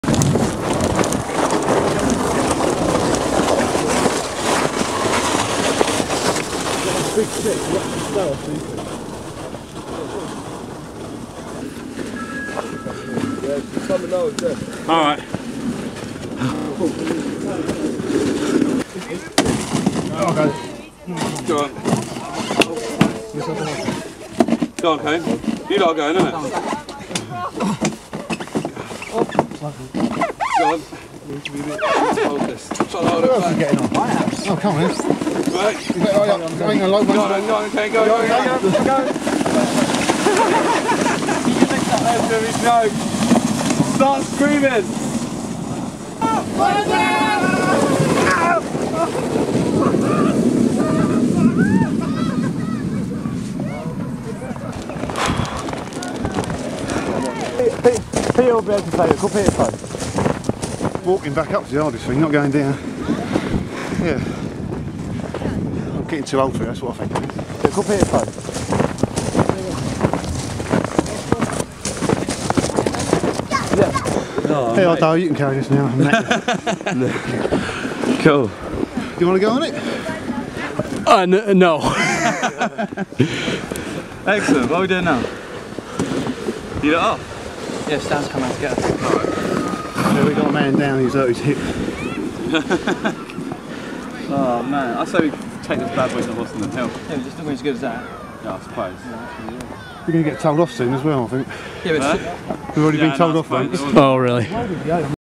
Snow sledding in London
Snow sledding down a narrow, icy path in Northwest London.